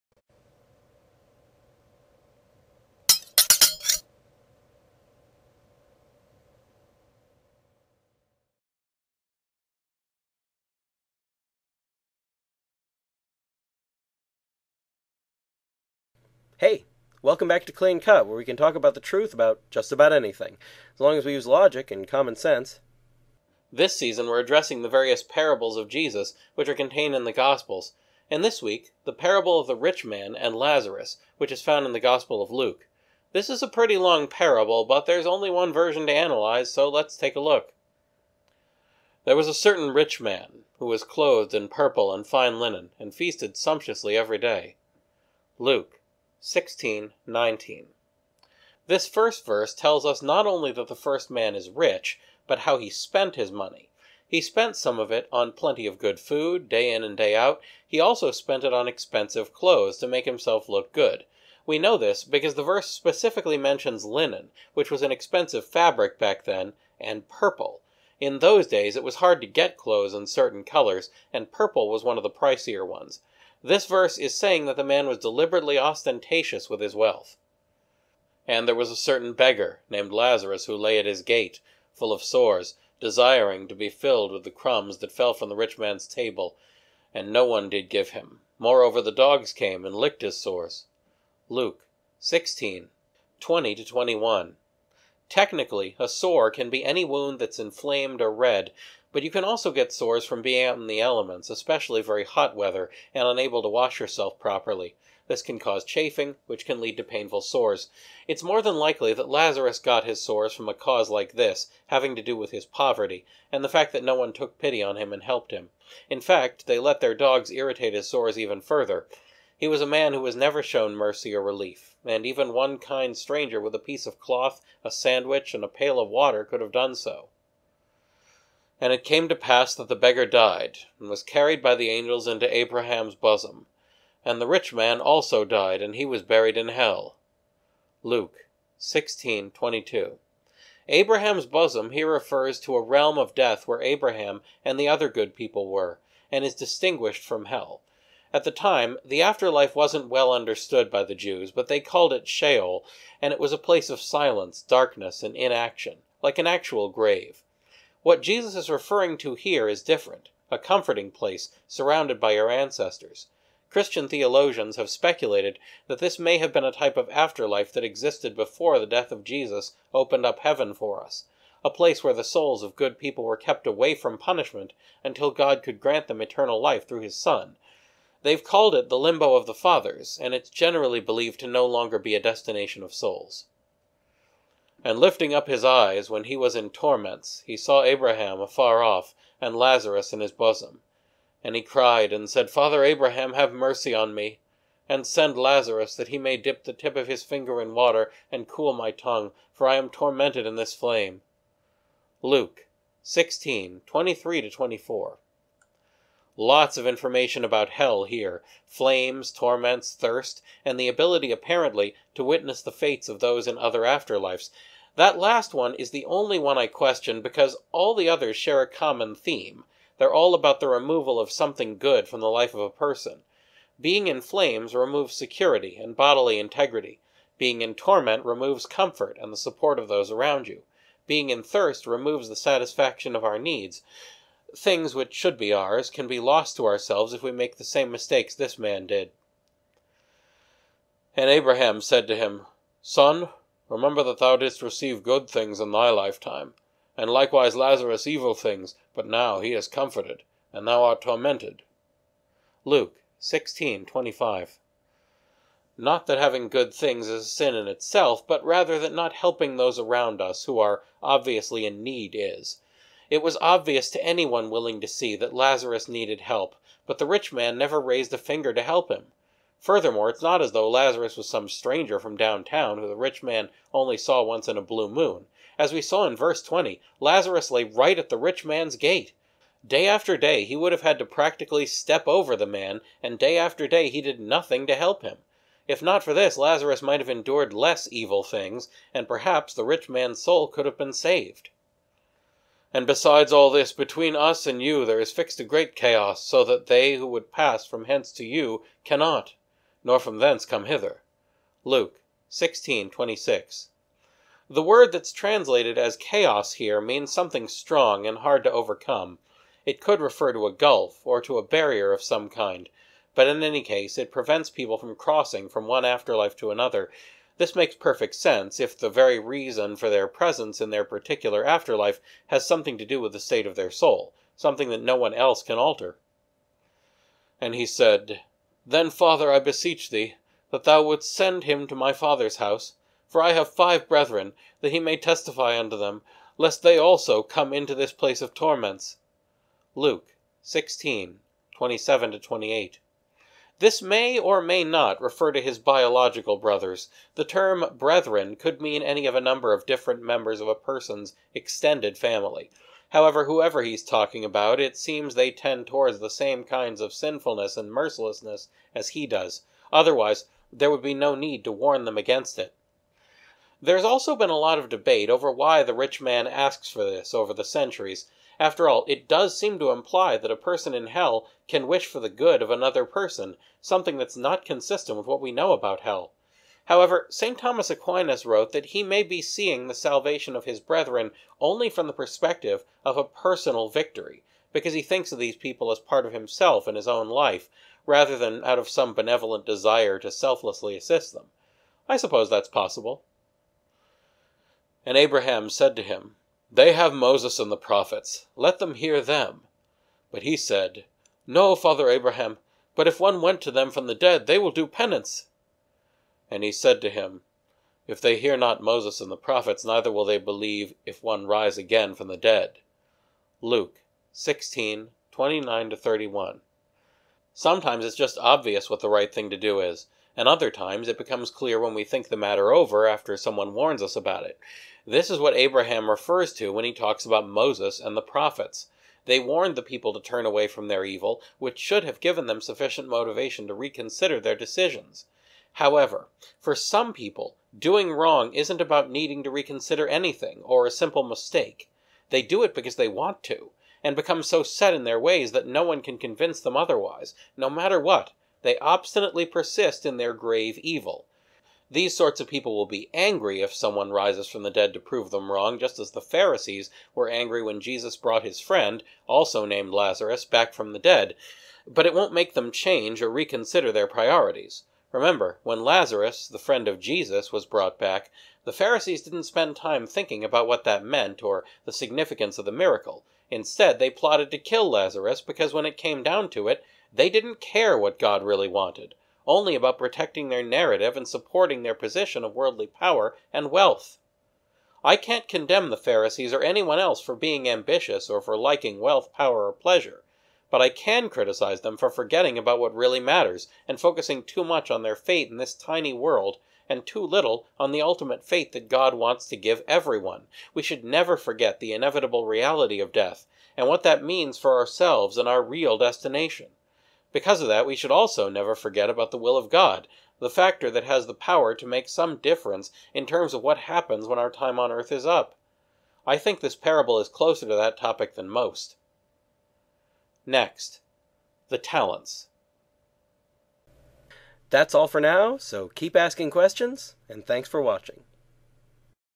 Why Was the Rich Man Doomed for Not Helping Lazarus?
How did the rich man know Lazarus?  How could the rich man have helped Lazarus?  What does his refusal to help say about the rich man?